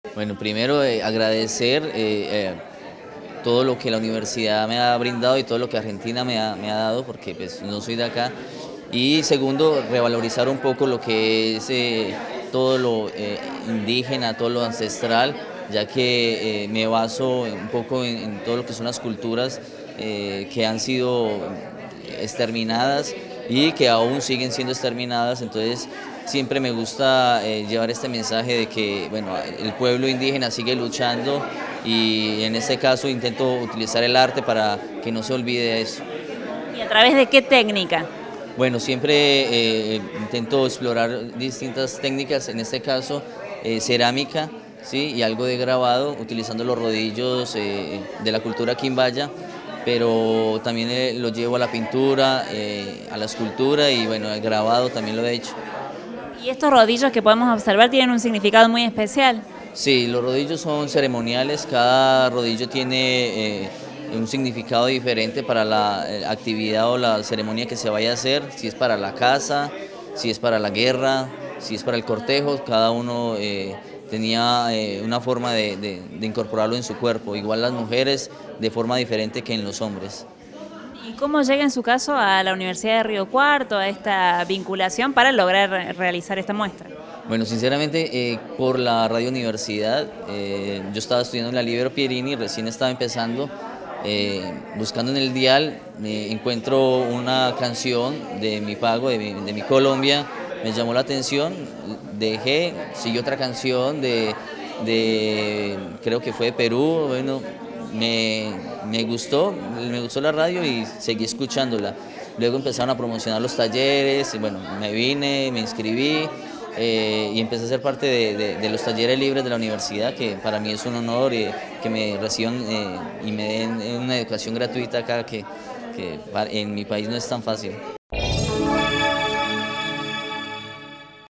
MUESTRA DE ARTE EN HUMANAS - ALBER GARCIA – ARTISTA PLÁSTICO
ALBER GARCIA – ARTISTA PLÁSTICO.  SE INAUGURO LA OBRA “ENTRE DOS TIERRAS” EN LA SALA DE CONSEJO DIRECTIVO

Facebook.com/uniriotv
Twitter.com/uniriotv
Instagram.com/uniriotv
Web - uniriotv.unrc.edu.ar
o envianos un email a
UniRio TV
Universidad Nacional de Río Cuarto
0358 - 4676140